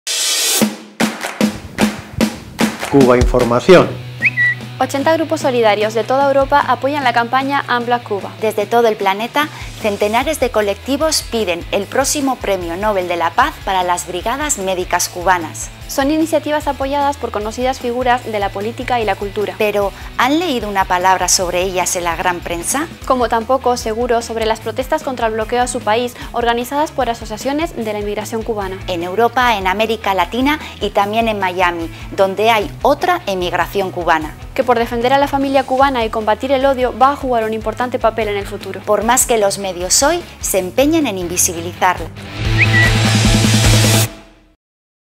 Cuba: solidaridad y emigración invisibles
80 grupos solidarios de toda Europa apoyan la campaña Unblock Cuba. Desde todo el planeta, centenares de colectivos piden el próximo Premio Nobel de la Paz para las Brigadas Médicas Cubanas.